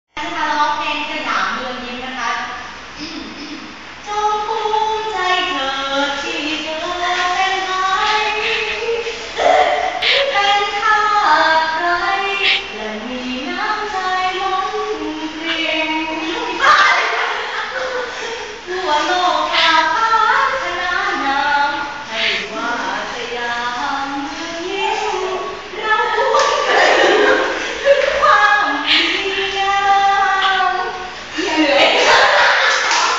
Crazyness in The last Exam day 1/2
2 vj สาว โชว์ความสามารถ
ครั้งนี้ เธอทั้งสองมาพร้อมกับเหล่าหางเครื่องของเธอ !!